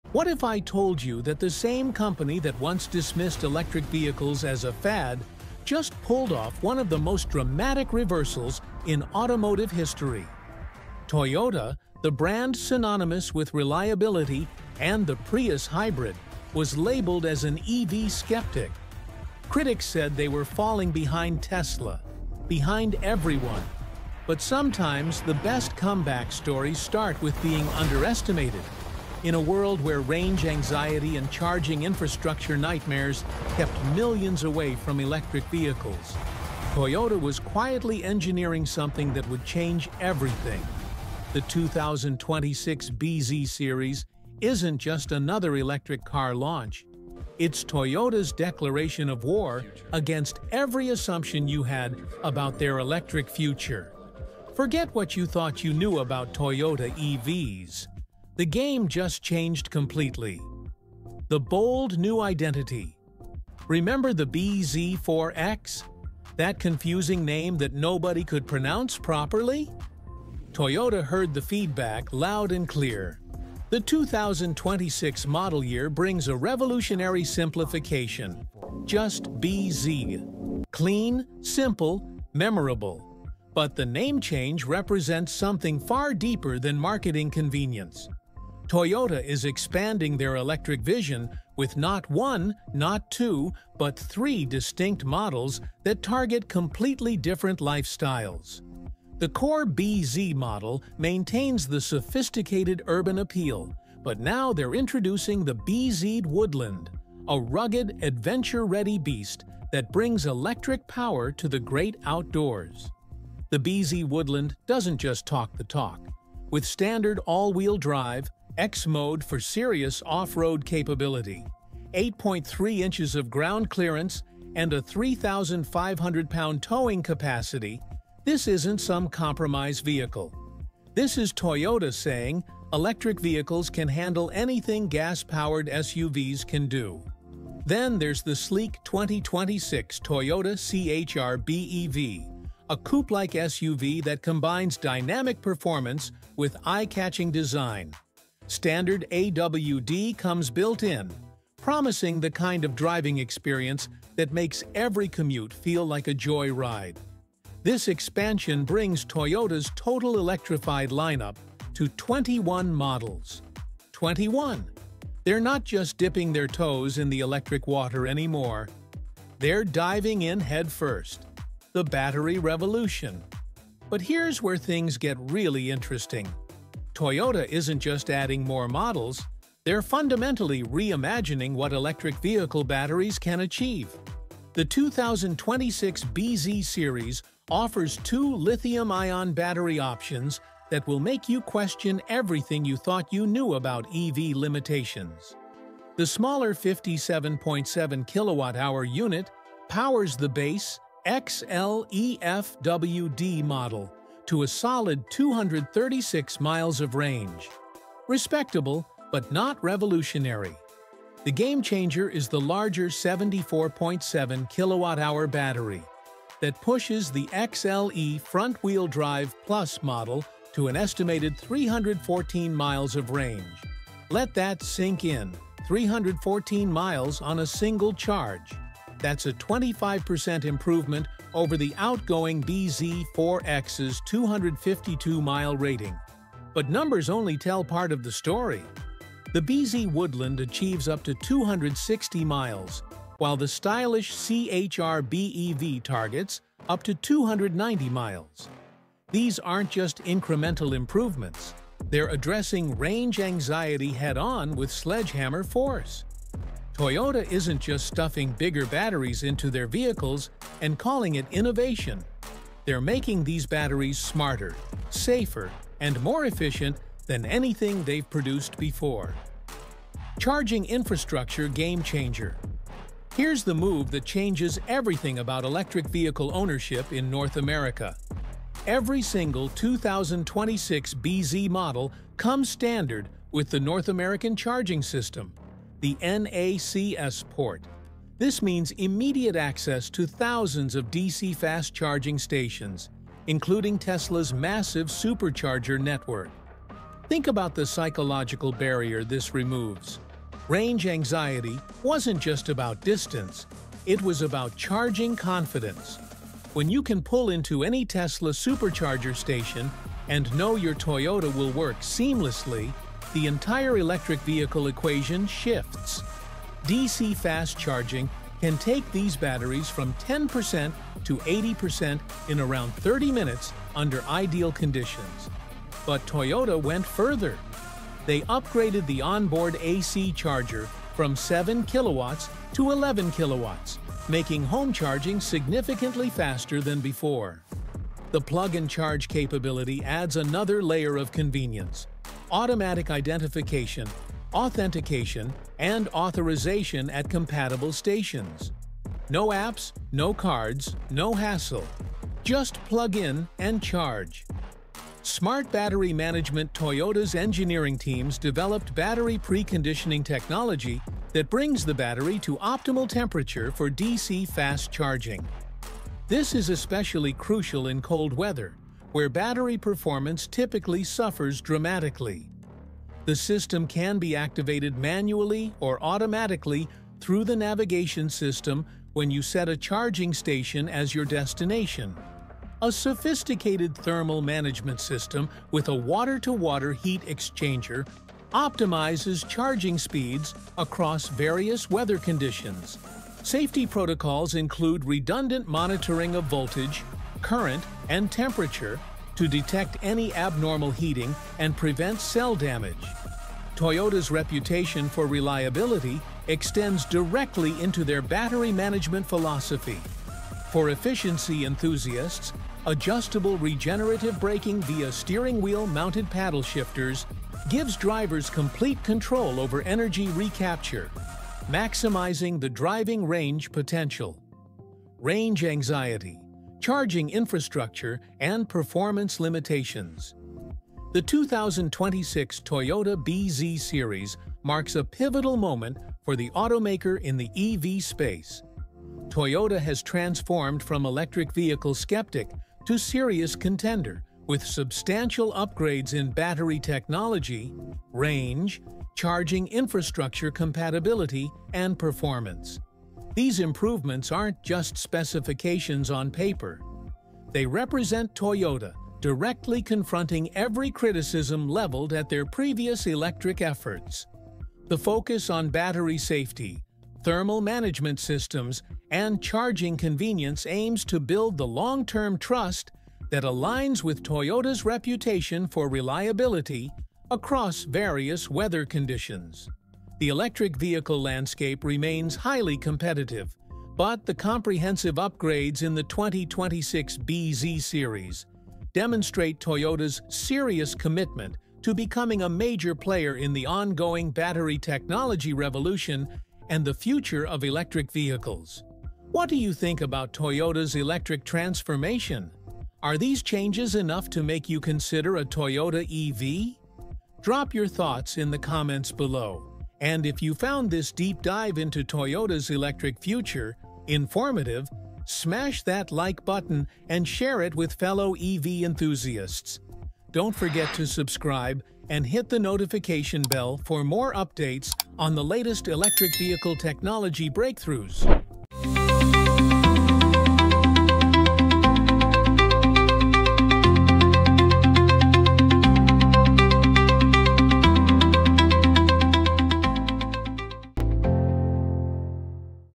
Is Toyota's 2026 BZ Series the EV Game-Changer We've Been Waiting For?
Toyota's EV Revolution: Unpacking the 2026 bZ Series Game-Changers!

The 2026 Toyota bZ series is here, and it's rewriting Toyota's electric vehicle story! This in-depth analysis examines the groundbreaking advancements that position Toyota as a formidable competitor in the electric vehicle (EV) market. We delve into the significantly improved battery technology, offering an impressive estimated range of up to 314 miles on the bZ XLE FWD Plus, directly tackling range anxiety.

Discover the all-new models joining the lineup:
- Toyota bZ: Simplified name, available in multiple trims with two battery options (57.7-kWh & 74.7-kWh).
- Toyota BZ Woodland: A new rugged all-electric SUV with standard AWD, X-MODE, 8.3 inches of ground clearance, and 3,500-pound towing capacity.
- Toyota C-HR BEV: A stylish, coupe-like SUV with standard AWD, offering dynamic performance.

Key Technology Upgrades Covered:
1. Enhanced Charging:
Standard NACS port for access to Tesla Superchargers & other DC fast chargers (10-80% in ~30 mins). Upgraded 11-kW onboard AC charger for faster home charging. Plug & Charge capability.

2. Advanced Battery Management:
Battery pre-conditioning for optimal DC fast charging, especially in cold weather. Sophisticated thermal management systems. Comprehensive safety systems with redundant monitoring.

3. Performance Boost:
AWD models now produce 338 hp, a 50% increase from previous models.

4. Competitive Stance:
See how the bZ series compares in range and charging against key competitors like the Hyundai Ioniq 5, Kia EV6, Ford Mustang Mach-E, and Volkswagen ID.4.

5. Future Tech:
Insights into Toyota's plans for solid-state batteries (aiming for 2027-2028), high-nickel cathodes, and anticipated EV battery cost reductions.

Join us as we explore Toyota's refined electric vehicle (EV) strategy, its impact on consumer adoption, and how the 2026 bZ series addresses previous criticisms to offer a compelling choice for EV buyers. Learn why these updates are crucial for Toyota's presence in the North American EV market.

What are your thoughts on the 2026 Toyota bZ lineup?